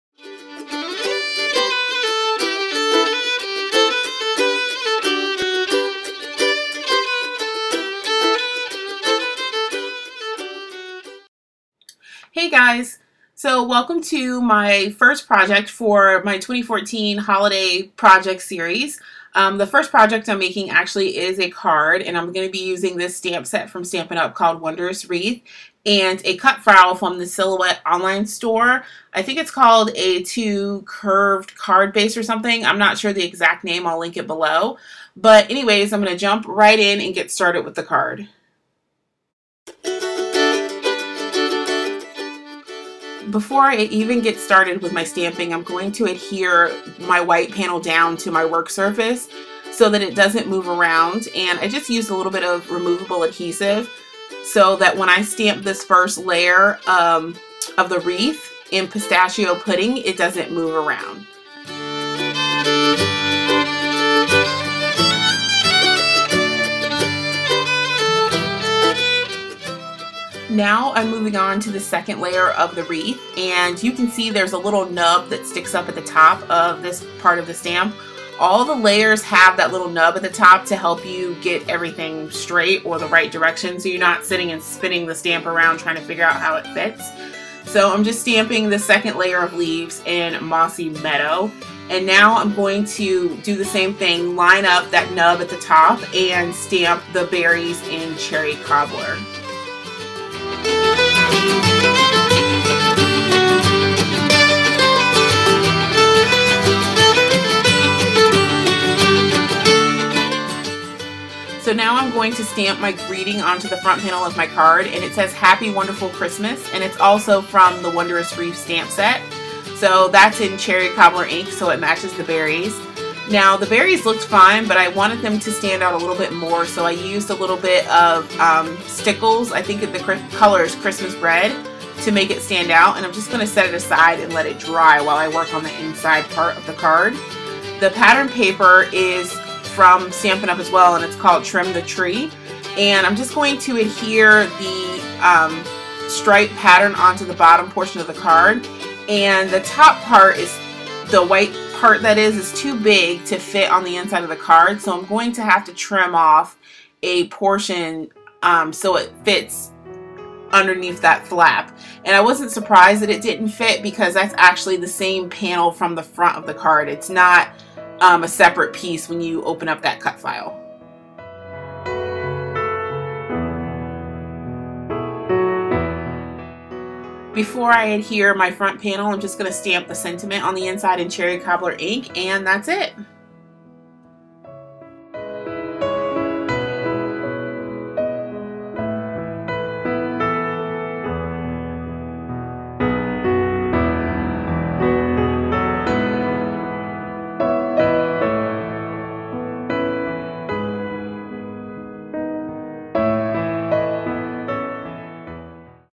Happy Wonderful Christmas Card | Holiday Project Series 2014
Hello All!

I'm kicking off my holiday crafting early this year! This is the first project in my 2014 Holiday Project Series! The plan is to make more than cards this year. Enjoy! =) (EXPAND to see Supplies Listed)